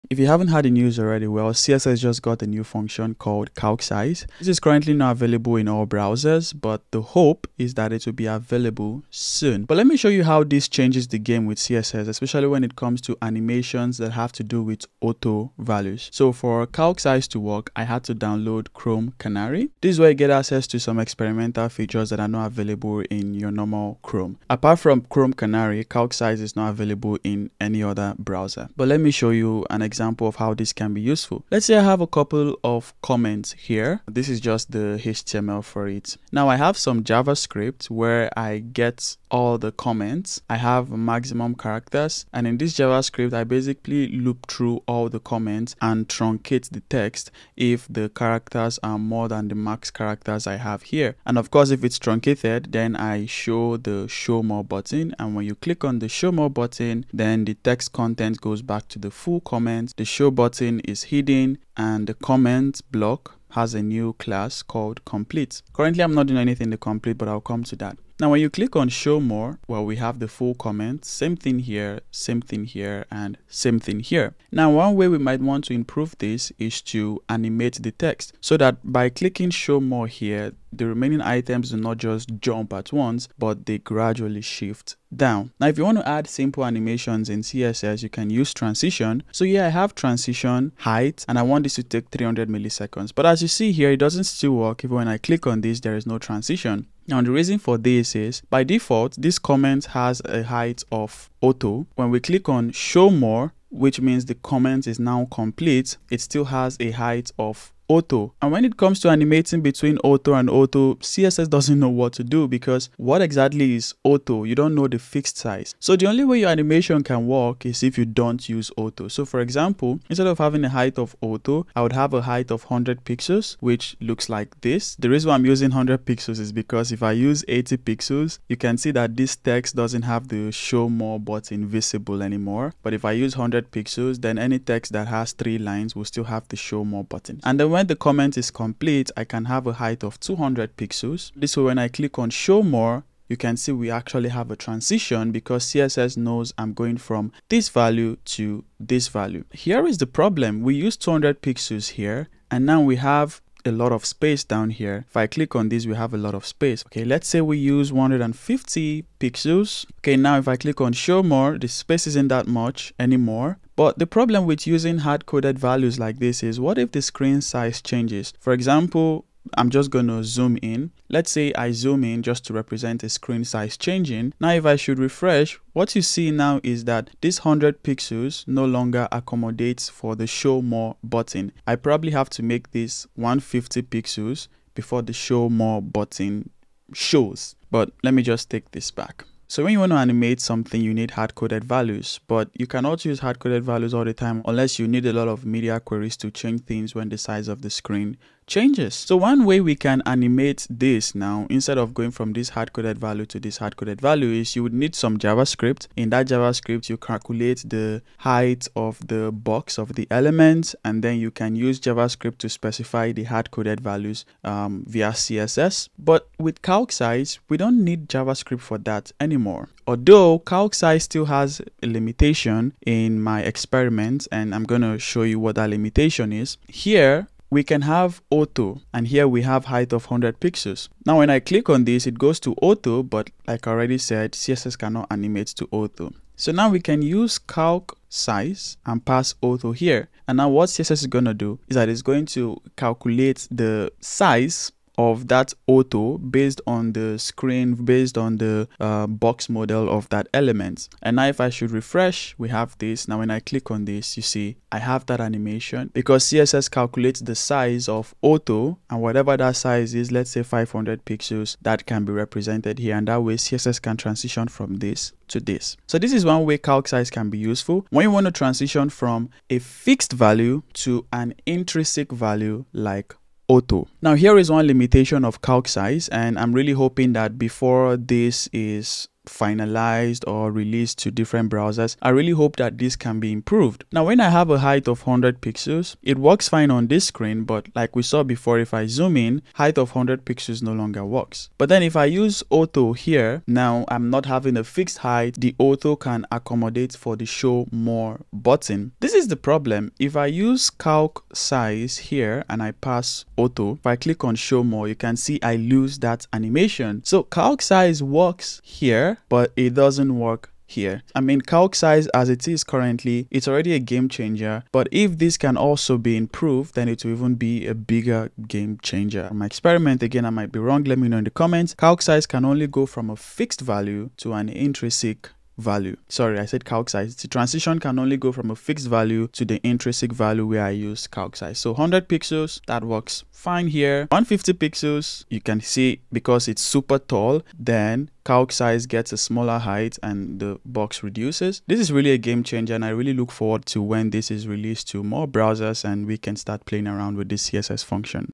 CSS CALC-SIZE - Game Changer & Limitations
Recently, CSS got a new function (in Chrome Canary), which is calc-size. This function makes it easy to animate auto values and in this video, I showed an example.